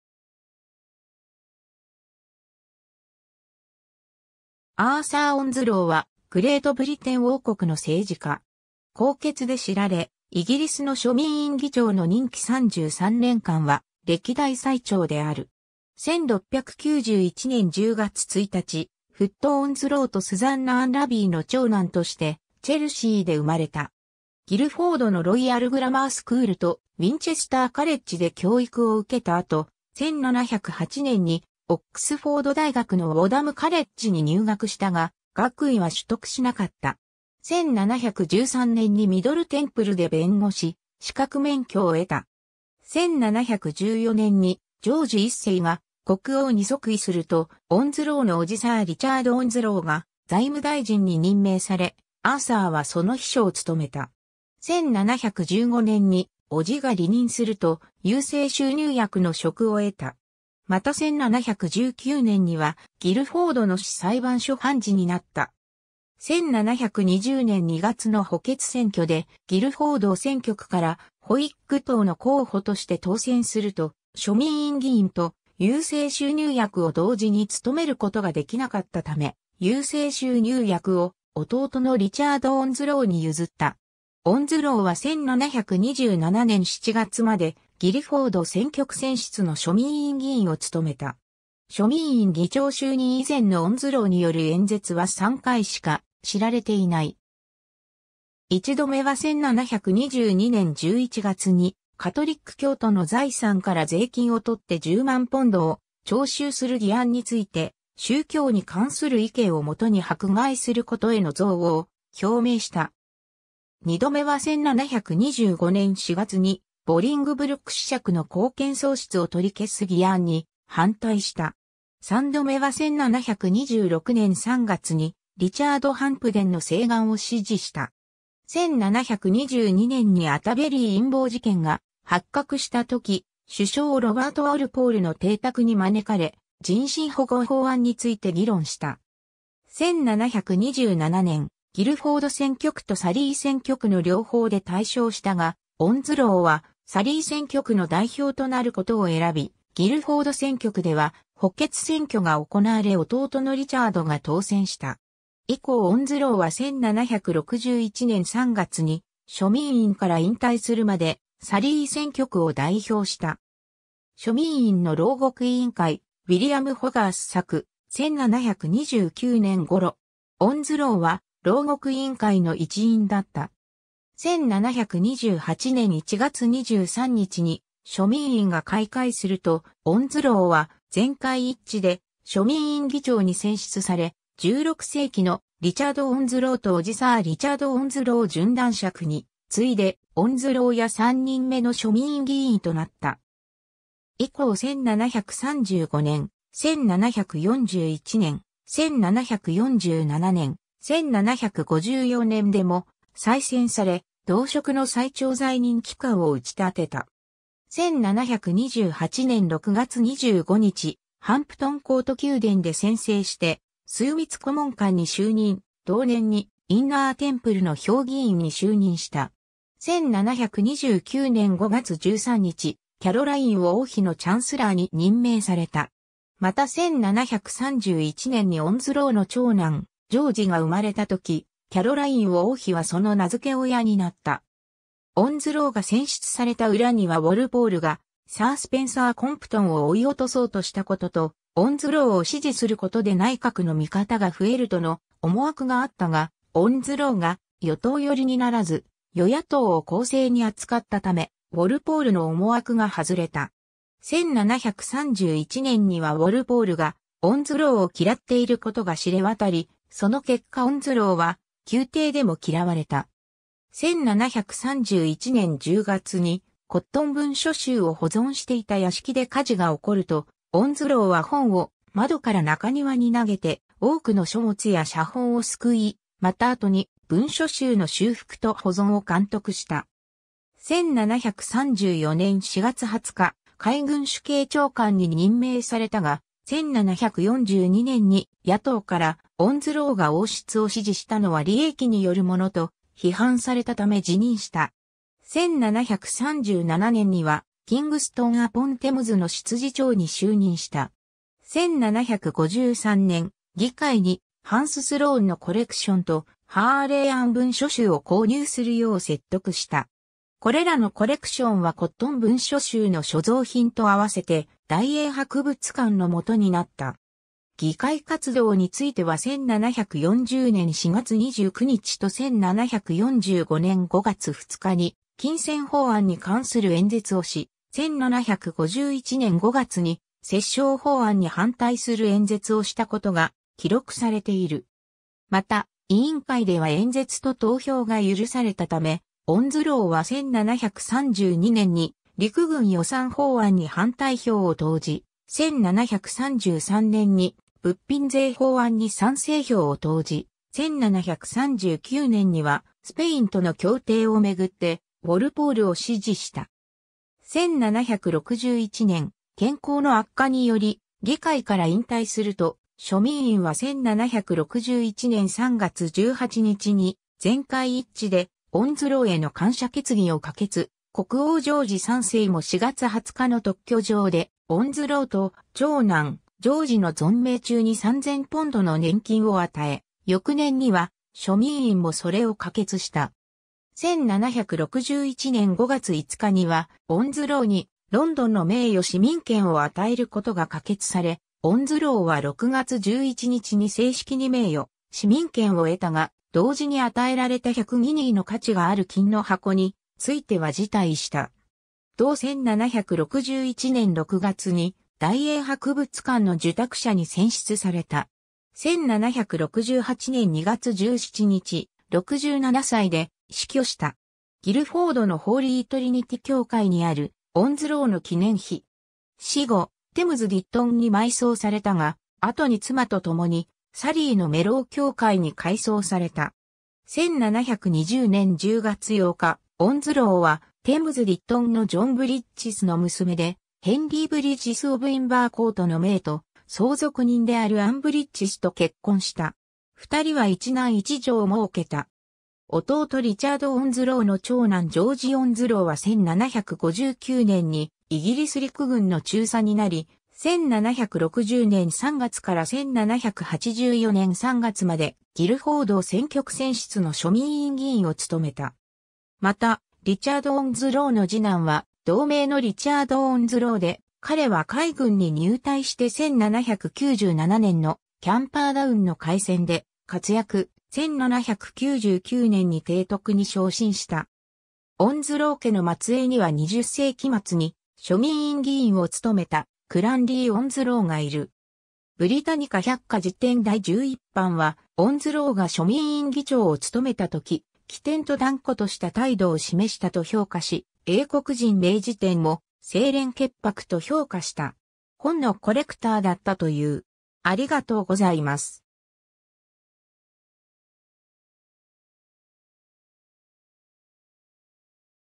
アーサー・オンズロー
イギリス・ホイッグ党の政治家
ウィンチェスター・カレッジ出身の人物
ミドル・テンプル出身の人物
アーサー・オンズロー（英語: Arthur Onslow PC、1691年10月1日 - 1768年2月17日）は、グレートブリテン王国の政治家。
高潔で知られ、イギリスの庶民院議長の任期33年間は歴代最長である。
 1691年10月1日、フット・オンズロー（Foot Onslow、1710年没）とスザンナ・アンラビー（Susanna Anlaby）の長男としてチェルシーで生まれた。
ギルフォードのロイヤル・グラマー・スクールとウィンチェスター・カレッジで教育を受けた後、1708年にオックスフォード大学のウォダム・カレッジに入学したが、学位は取得しなかった。
1713年にミドル・テンプルで弁護士資格免許を得た。
 1714年にジョージ1世が国王に即位すると、オンズローの伯父サー・リチャード・オンズローが財務大臣に任命され、アーサーはその秘書を務めた。
1715年に伯父が離任すると、郵政収入役の職を得た。
また1719年にはギルフォードの市裁判所判事になった。
1720年2月の補欠選挙でギルフォード選挙区からホイッグ党の候補として当選すると、庶民院議員と郵政収入役を同時に務めることができなかったため、郵政収入役を弟のリチャード・オンズローに譲った。
オンズローは1727年7月までギルフォード選挙区選出の庶民院議員を務めた。
 庶民院議長就任以前のオンズローによる演説は3回しか知られていない。
1度目は1722年11月にカトリック教徒の財産から税金をとって10万ポンドを徴収する議案について、「宗教に関する意見をもとに迫害することへの憎悪を表明した」。
2度目は1725年4月にボリングブルック子爵の公権喪失を取り消す議案に反対した。
3度目は1726年3月にリチャード・ハンプデンの請願を支持した。
1722年にアタベリー陰謀事件が発覚したとき、首相ロバート・ウォルポールの邸宅に招かれ、人身保護法案について議論した。
 1727年、ギルフォード選挙区とサリー選挙区の両方で大勝したが、オンズローはサリー選挙区の代表となることを選び、ギルフォード選挙区では補欠選挙が行われ弟のリチャードが当選した。
以降オンズローは1761年3月に庶民院から引退するまでサリー選挙区を代表した。
 『庶民院の牢獄委員会』、ウィリアム・ホガース作、1729年頃。
オンズローは牢獄委員会の一員だった。
 1728年1月23日に庶民院が開会すると、オンズローは全会一致で庶民院議長に選出され、16世紀のリチャード・オンズローと伯父サー・リチャード・オンズロー準男爵についでオンズロー家3人目の庶民院議員となった。
以降1735年、1741年、1747年、1754年でも再選され、同職の最長在任期間（33年）を打ち立てた。
1728年6月25日、ハンプトン・コート宮殿で宣誓して枢密顧問官に就任、同年にインナー・テンプルの評議員に就任した。
1729年5月13日、キャロライン王妃のChancellorに任命された。
また1731年にオンズローの長男ジョージが生まれたとき、キャロライン王妃はその名付け親になった。
 オンズローが選出された裏にはウォルポールがサー・スペンサー・コンプトンを追い落とそうとしたことと、オンズローを支持することで内閣の味方が増えるとの思惑があったが、オンズローが与党寄りにならず与野党を公正に扱ったためウォルポールの思惑が外れた。
1731年にはウォルポールがオンズローを嫌っていることが知れ渡り、その結果オンズローは宮廷でも嫌われた。
 1731年10月にコットン文書集を保存していた屋敷で火事が起こると、オンズローは本を窓から中庭に投げて多くの書物や写本を救い、また後に文書集の修復と保存を監督した。
1734年4月20日、海軍主計長官に任命されたが、1742年に野党からオンズローが王室を支持したのは利益によるものと批判されたため辞任した。
1737年にはキングストン・アポン・テムズの執事長に就任した。
1753年、議会にハンス・スローンのコレクションとハーレーアン文書集を購入するよう説得した。
これらのコレクションはコットン文書集の所蔵品と合わせて大英博物館の元になった。
 議会活動については1740年4月29日と1745年...